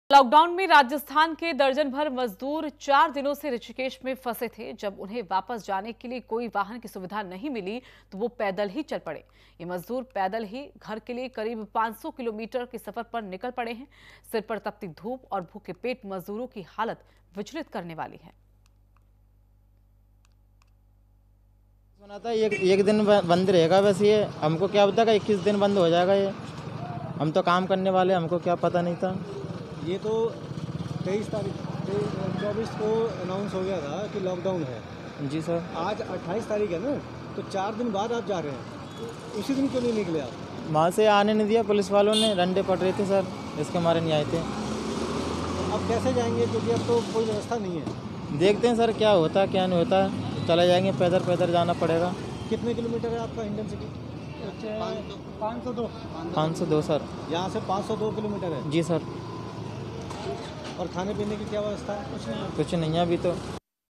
लॉकडाउन पर भारी पड़ रहा भूख, चार दिनों से ऋषिकेश में फंसे कई मजदूर R9TV
लॉकडाउन में राजस्थान के दर्जनभर मजदूर चार दिनों से ऋषिकेश में फंसे थे। जब उन्हें वापस जाने के लिए कोई वाहन की सुविधा नहीं मिली तो वो पैदल ही चल पड़े. ये मजदूर पैदल ही घर के लिए करीब 500 किलोमीटर के सफर पर निकल पड़े हैं।